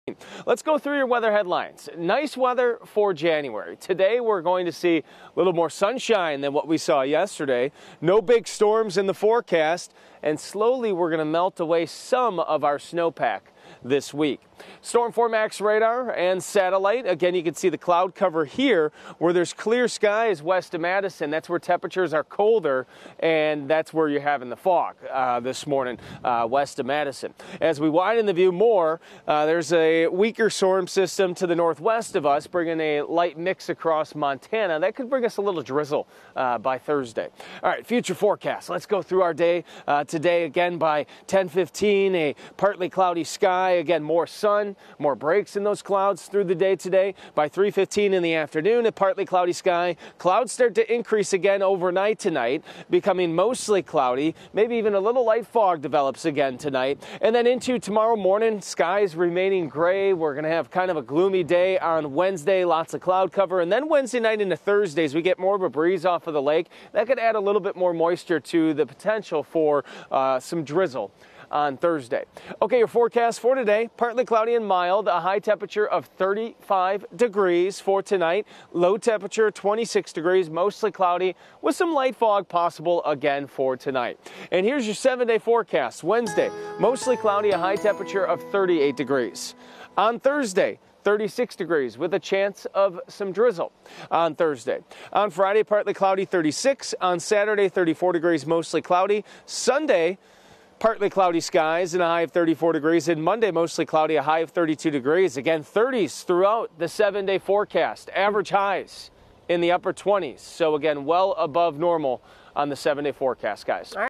Above normal weather in store for SE Wisconsin
We're entering the Dead of Winter and our weather this week will be far from it. Each day this week will be above normal.